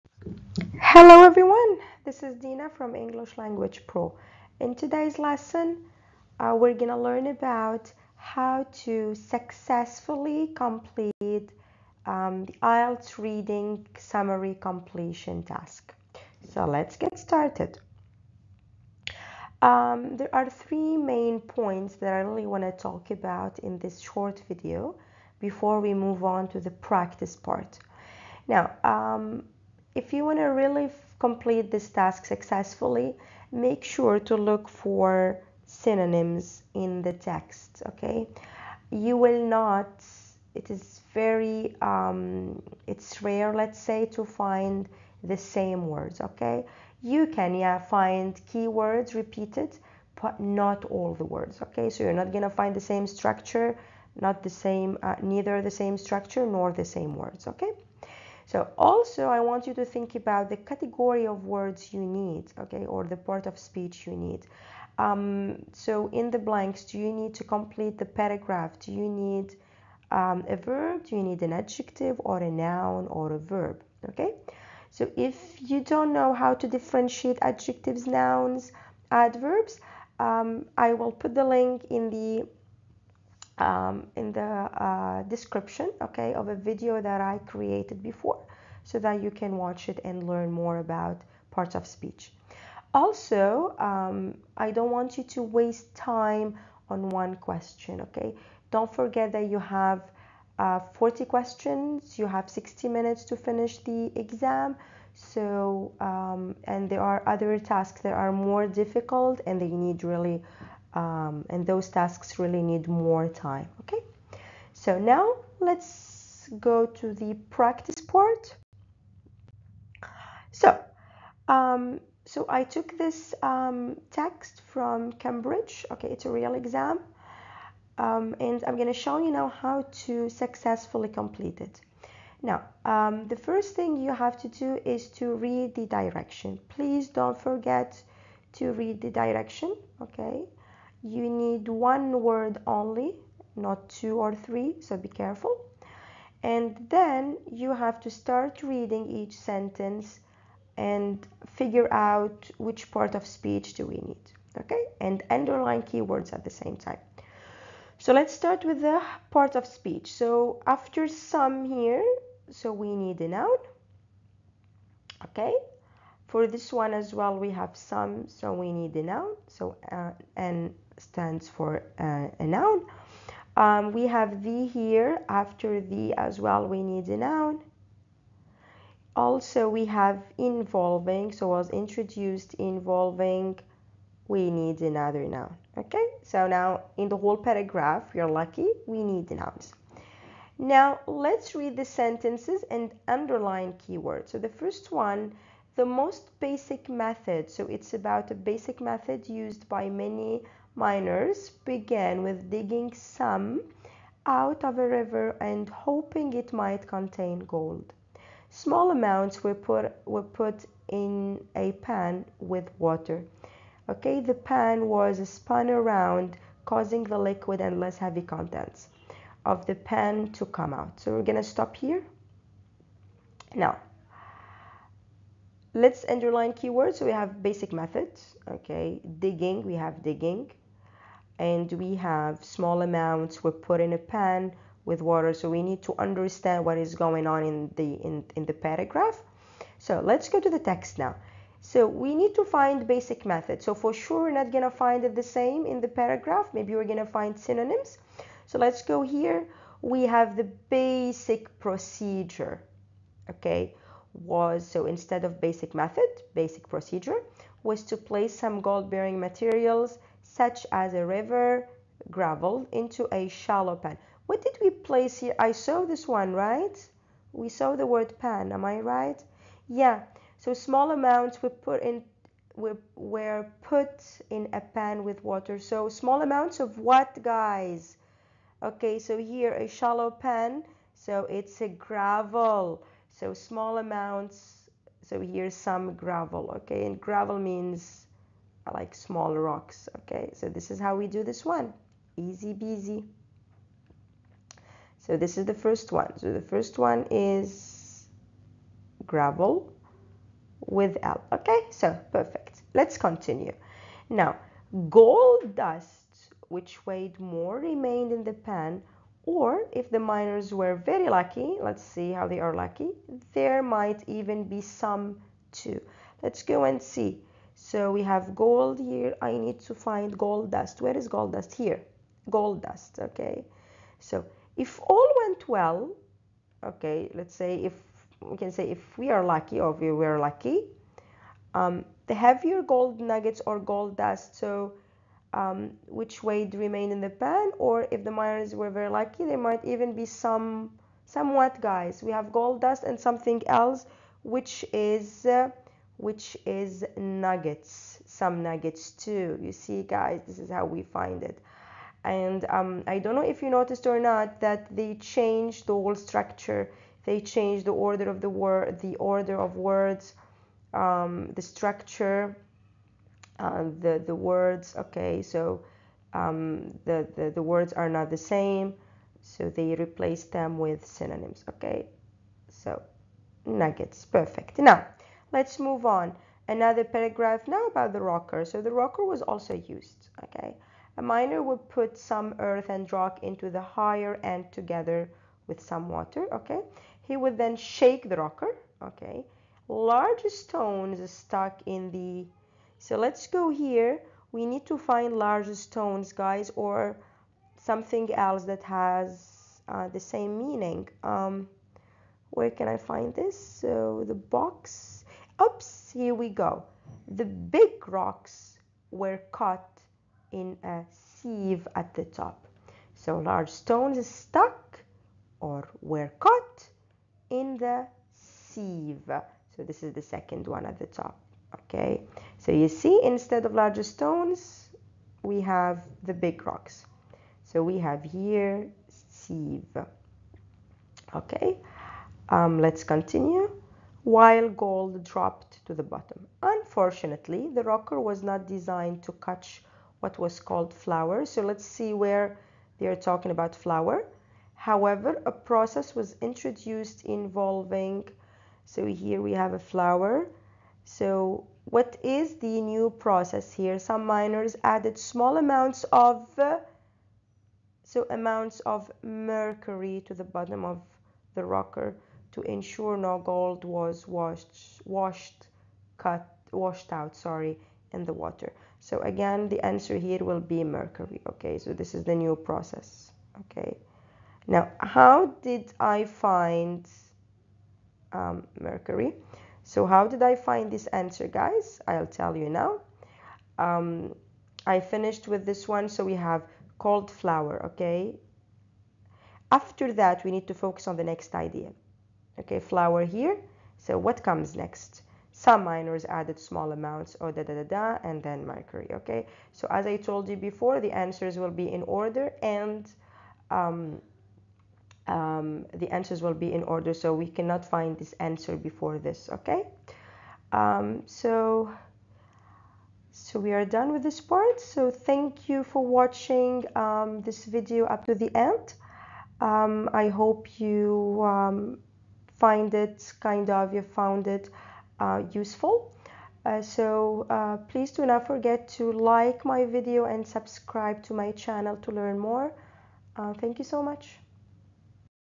IELTS Reading -Summary Completion
In this lesson, you will learn a simple method that will help you to answer summary completion questions in a more effective way.